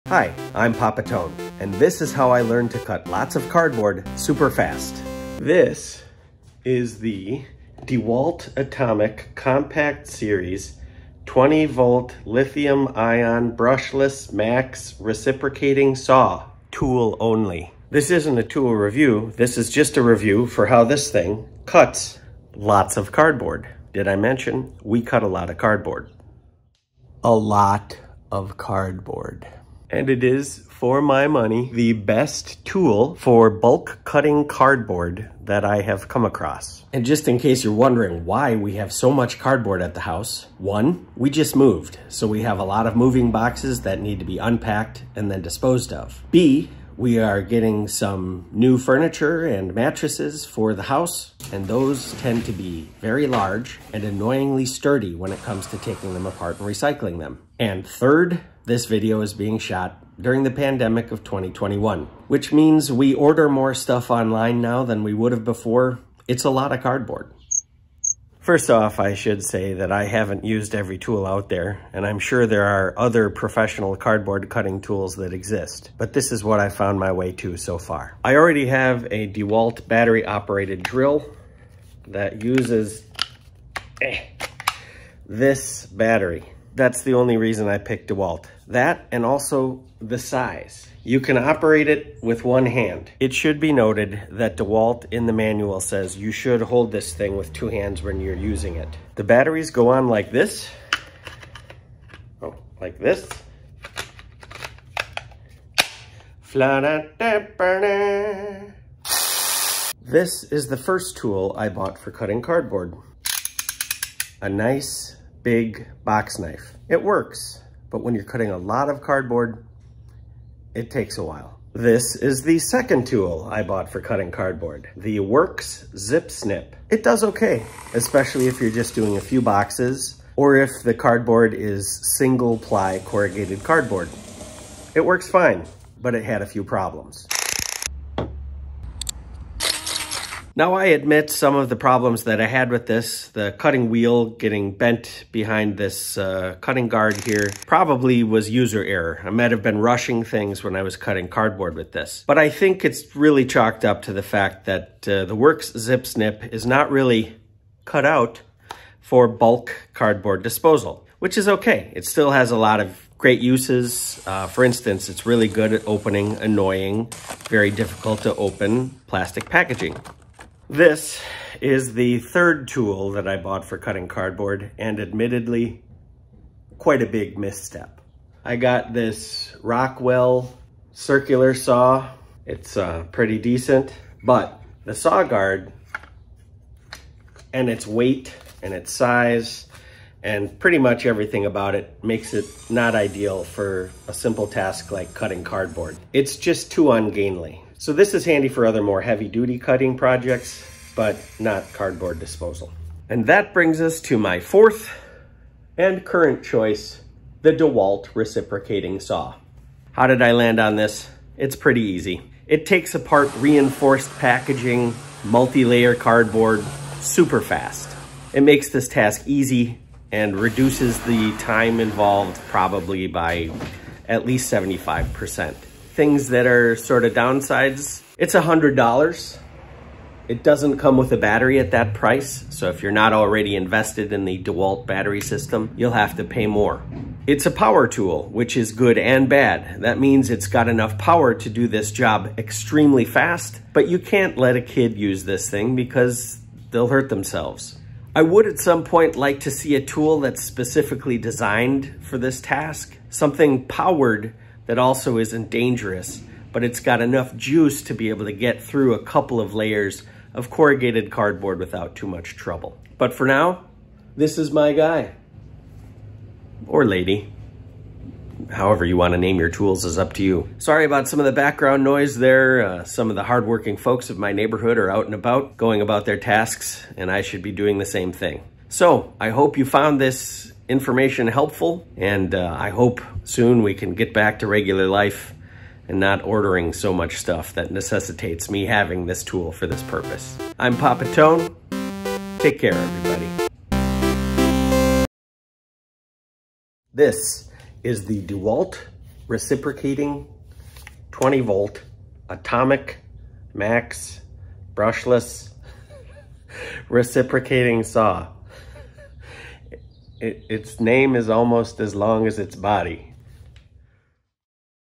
How To Cut LOTS of Cardboard FAST - Box Knife vs Worx ZipSnip vs DeWalt Saw
How much time do you spend cutting cardboard every week? Here at Camden House, I used to spend at least an hour or two breaking down boxes on a weekly basis. Grocery boxes. Amazon boxes. Furniture boxes. Target boxes. Some of it is due to a recent move, some of it is because of the pandemic, and some of it is because we just have more packaging around now than we ever did. In this video, I break down (haha) a few of the different tools I have used along the way, trying always to find a way to make this weekly task quicker and more efficient.

Hopefully...it won't always be this way. Hopefully, the world will open up and we won't need so much stuff shipped to us. But for now, here in late 2021, this is how it is. Might as well make it work.

Is there a tool you use for this task that I don't know about? Let me know what it is. For instance, I'd like to find a safer way to do this, so that I can put my kids to work! XD

This video is not sponsored in any way by any of the tools whose labels and packaging are featured prominently in this video. (I wish.)

Find me at PapatonePictures.com if you have ideas or things/tools/fully operational spaceships you would like me to take a look at on camera.

PapaTone aka Tony Teofilo